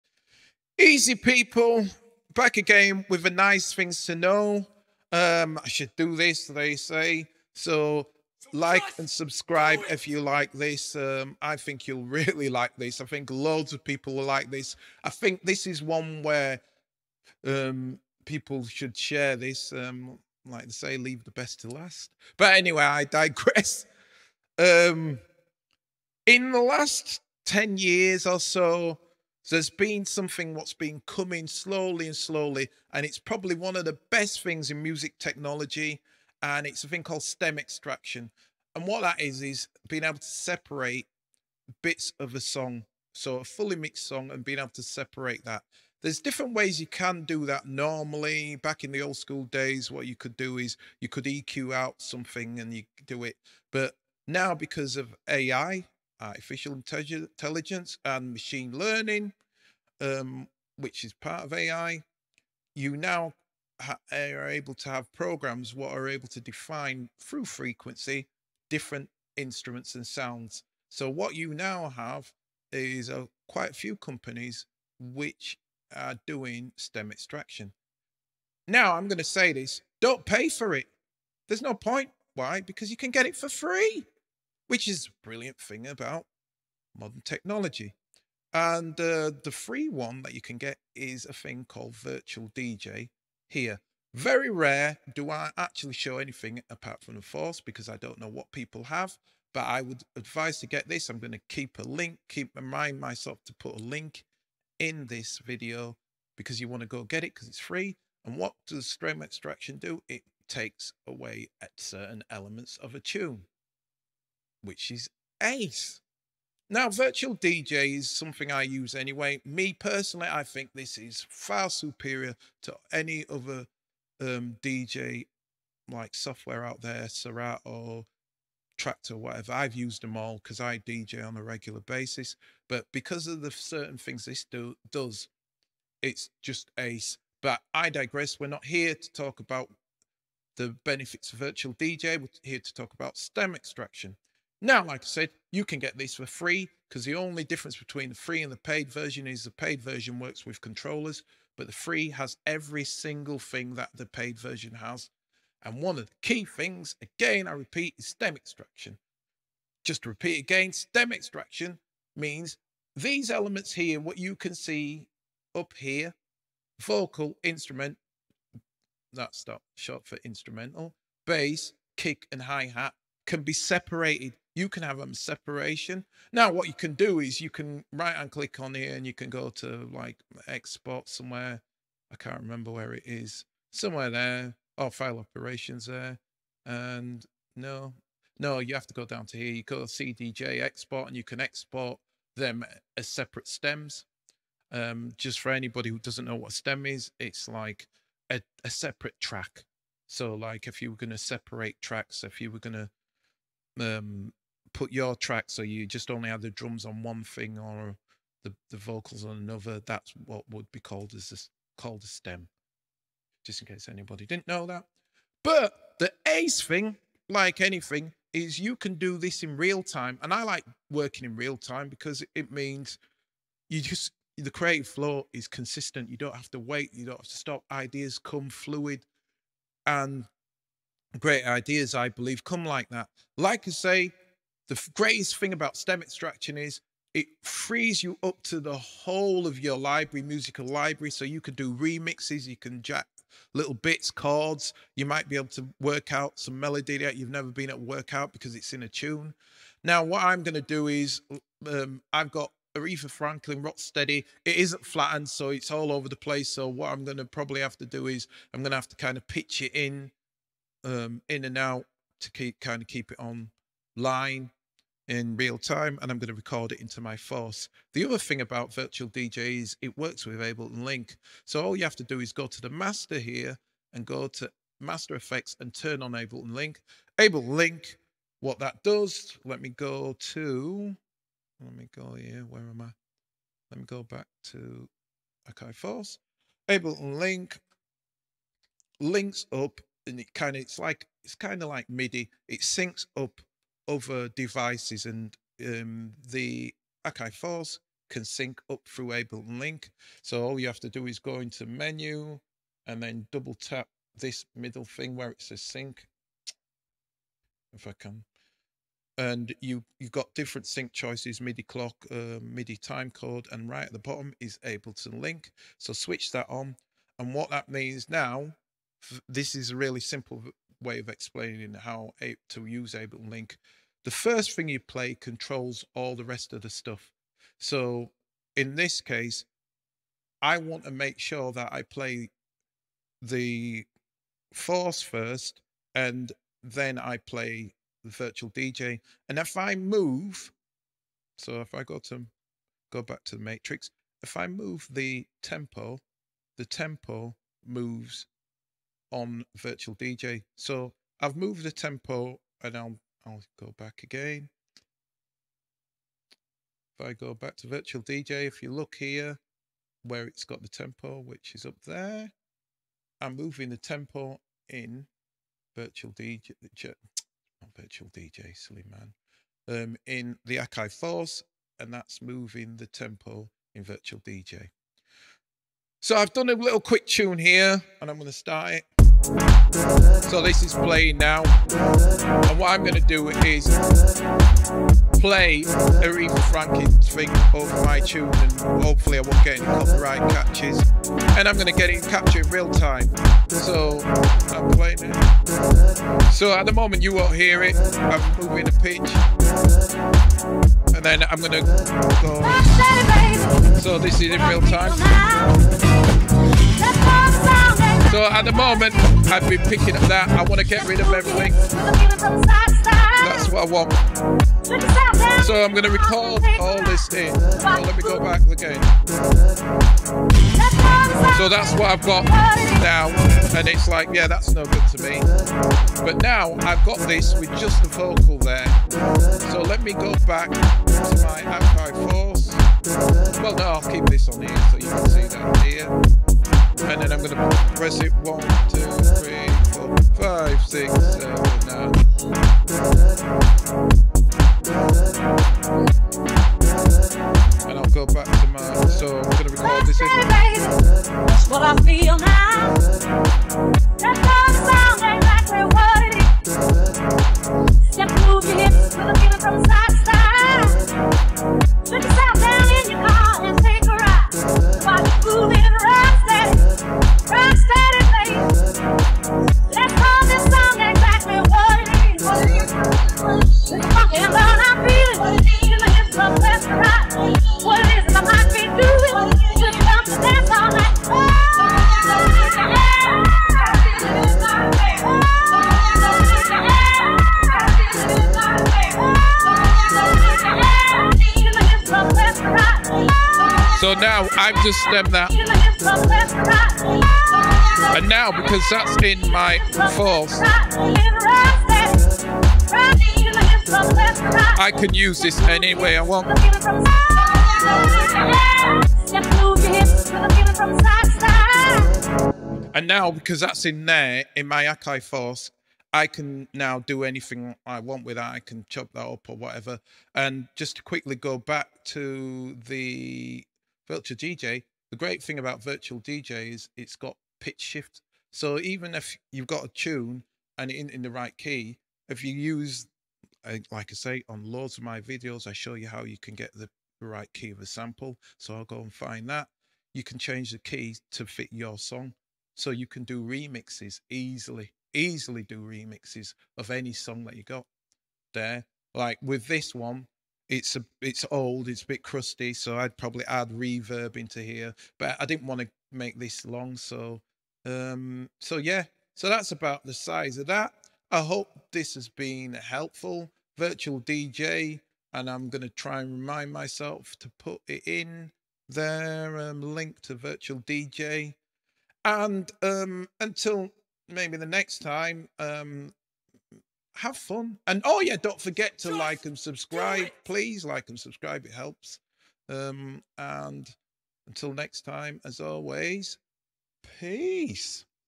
Nice Things to Know! - TUNE STRIPPING!
Easy, this is a tutorial for Stem Extraction techniques on the Akai Force.

Have fun!